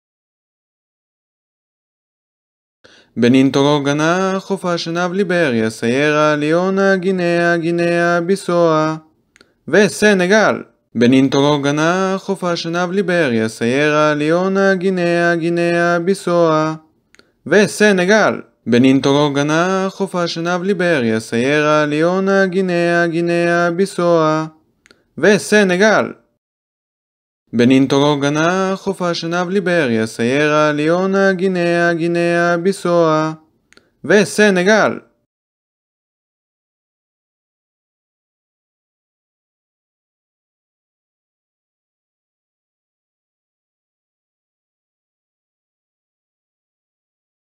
שירת זכרון בנין טוגו גאנה ושאר המדינות של החוף הדרום מערבי של אפריקה
שיר שיעזור לכם לזכור את המדינות שבין ניגריה וסנגל שאתם אף פעם לא מצליחים לזכור את הסדר שלהן במפה או איפה הן נמצאות, לפני שהמצאתי של השיר הזה ידעתי שליבריה נמצאת איפה שהוא שם אבל לא יכולתי להצביע במדויק עלייה במפה